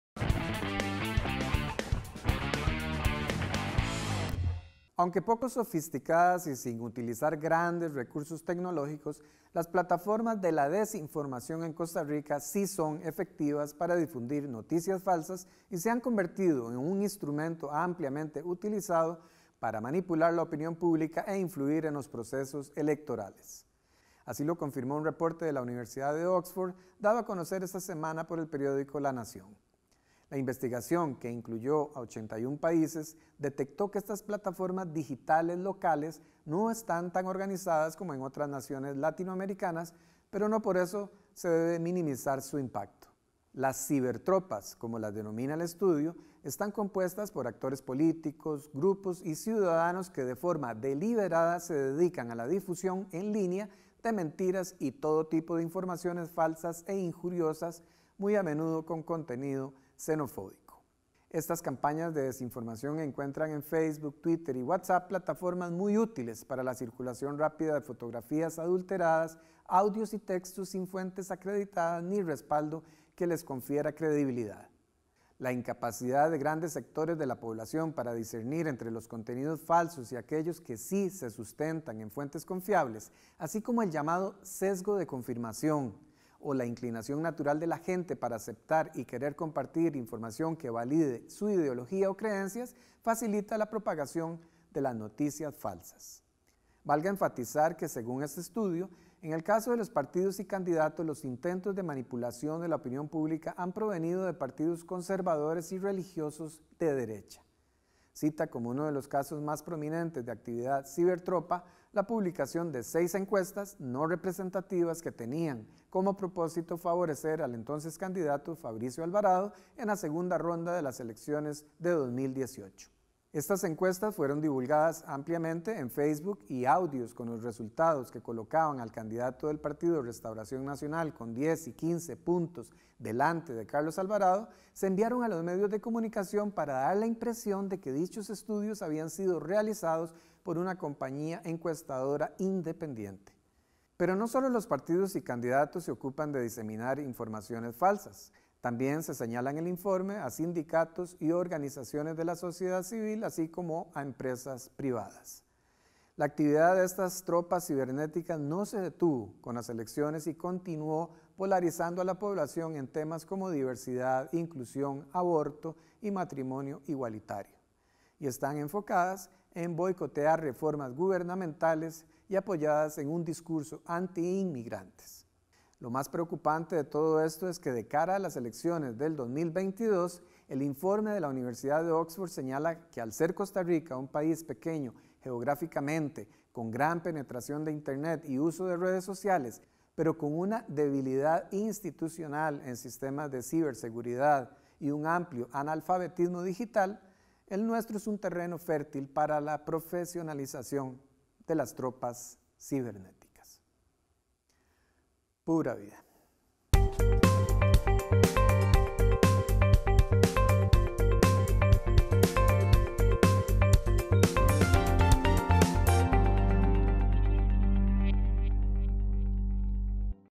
La Otra Noticia: Cibertropas de desinformación
Según un estudio de la Universidad de Oxford, estamos ante la amenaza de que políticos y troles de internet utilicen plataformas de desinformación para afectar elecciones del 2022.